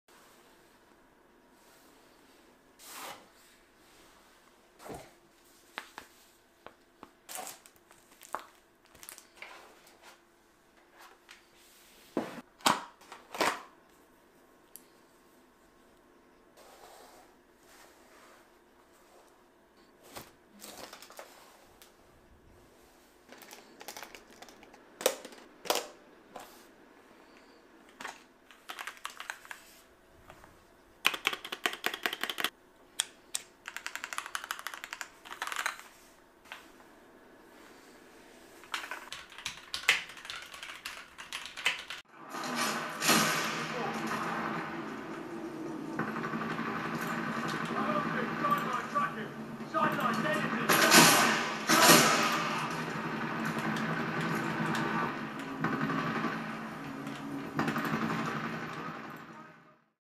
A perfect Travel Partner - Lofree Wanderfree Keyboard
Lofree Wanderfree is a perfect compact travel style gaming keyboard with its own suitcase.